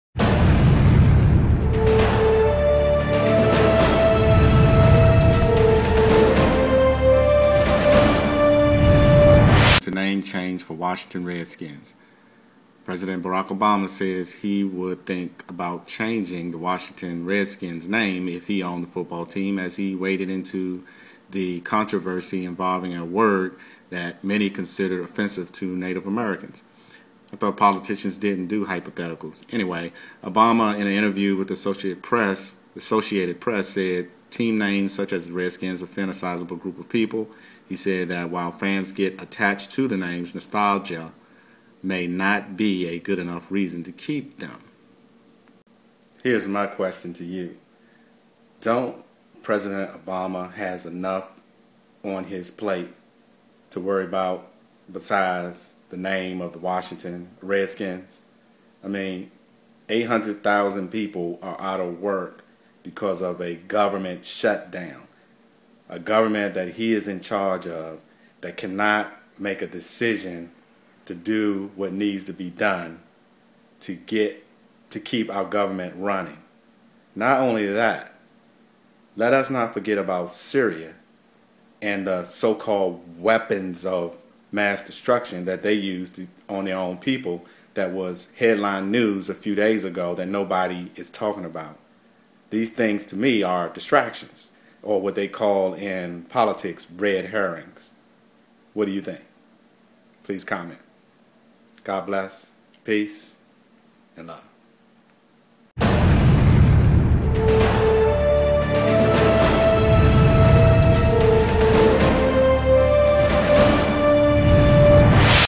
Obama's Take On Redskin's Team Name
Obama's take on Redskins's team name.

OK, why is Syria's dirty bombs not important any more and can we please get the government up and running again!